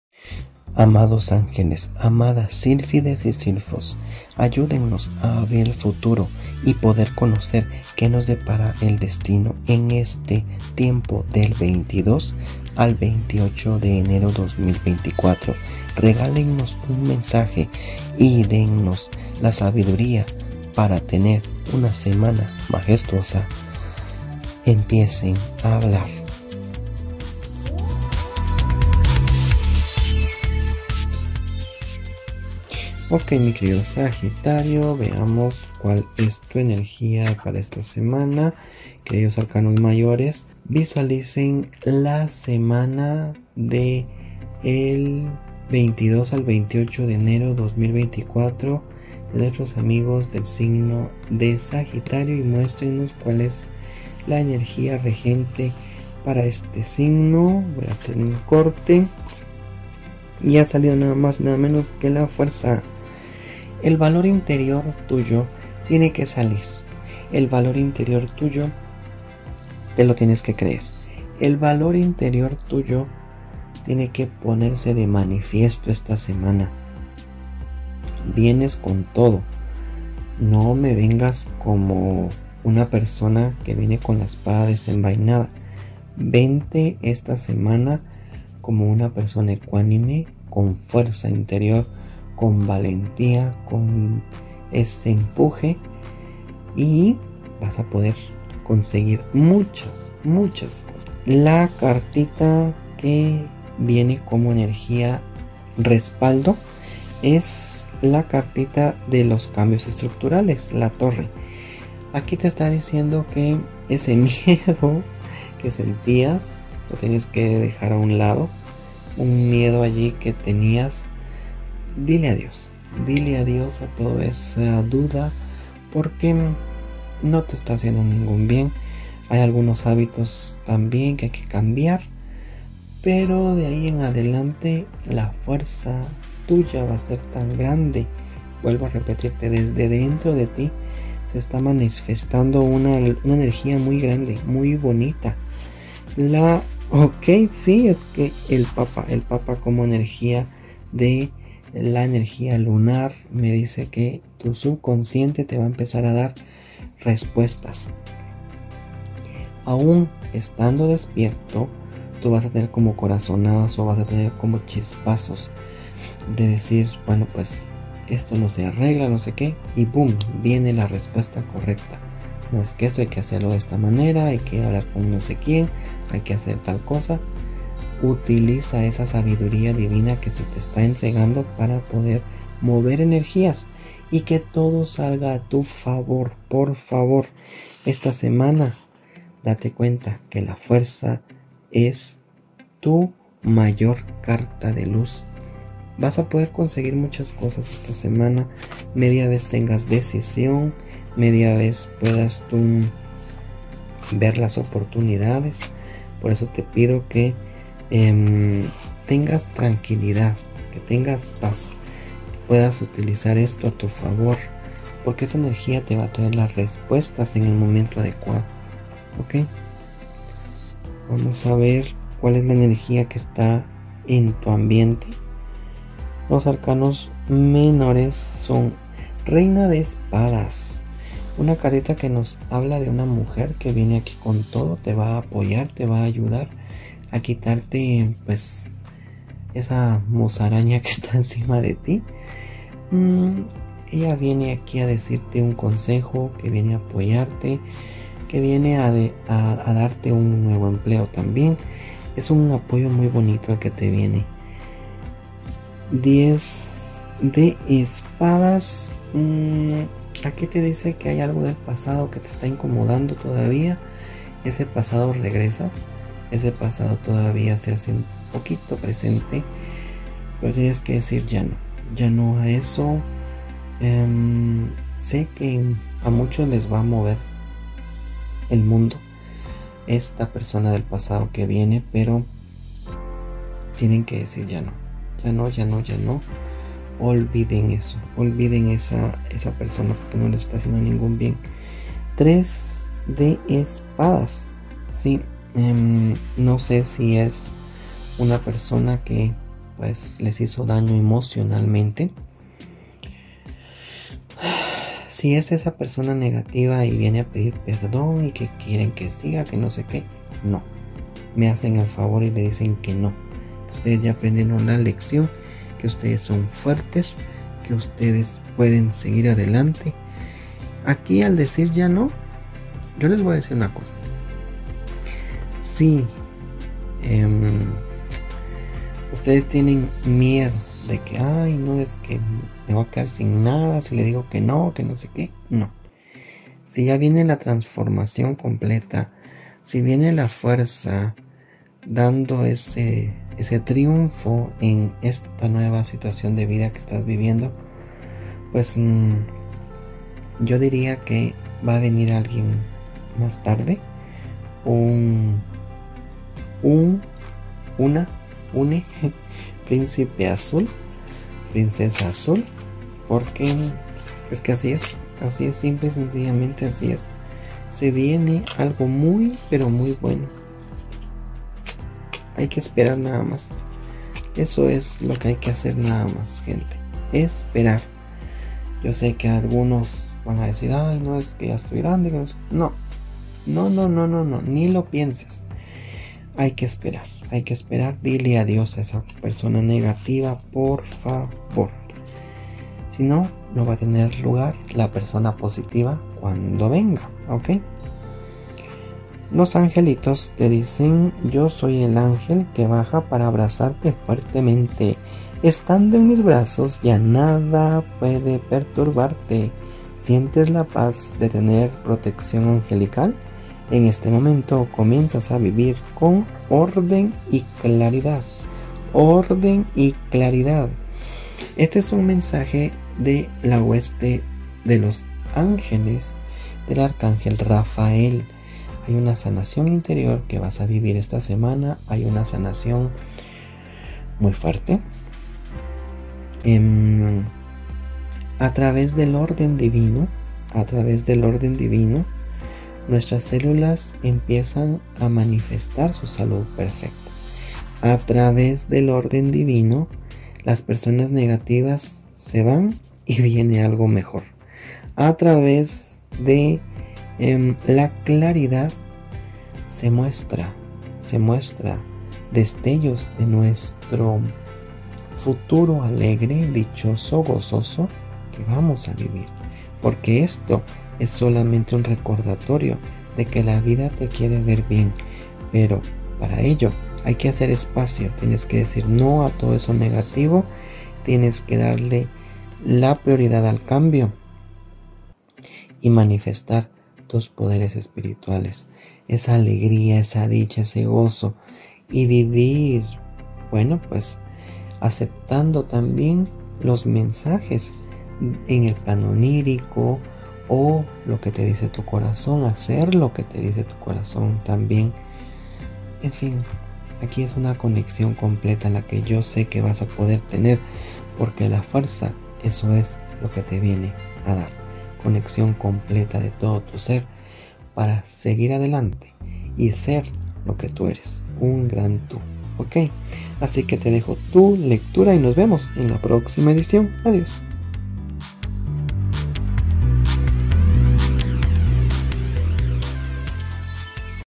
🌙✨ SAGITARIO: Revelando las Energías de la Semana del 22 al 28 de enero de 2024 🔮✨
Hola, amantes de la energía y la magia! ✨ En este nuevo viaje a través de las cartas, exploramos las energías que nos rodean durante la semana del  del 22 al 28 de enero de 2024. ¡Prepárense para descubrir los misterios que el universo tiene para revelarnos!

Comenzamos nuestra travesía sacando un Arcano Mayor para descubrir la energía regente de este nuevo tiempo. Esta carta nos dará las claves principales que marcarán el tono de la semana. 🌈

Como complemento, veremos el arcano de la energía respaldo que muestra más detalle de por donde se mueve la energía regente. También daremos un vistazo a la energía 🌙✨ lunar, que es ese arcano mayor que nos marca la energía que está allí empujando que todo se de según el momento que estás transitando.

Nuestros protectores celestiales nos envían un mensaje amoroso y nos obsequian un regalo especial a través del Oráculo de los Ángeles. Estos mensajeros divinos nos guiarán con su luz y nos brindarán apoyo en nuestro camino. 👼🏻✨

Cerramos esta experiencia mágica con reflexiones finales y bendiciones para todos aquellos que nos han acompañado en este viaje espiritual. Que esta semana esté llena de amor, paz y abundancia para cada uno de ustedes. 🙏🏼💖

¡No olviden compartir sus experiencias y pensamientos en los comentarios! Que la magia de esta semana los envuelva con su luz. ¡Hasta la próxima, queridos espectadores! 🌟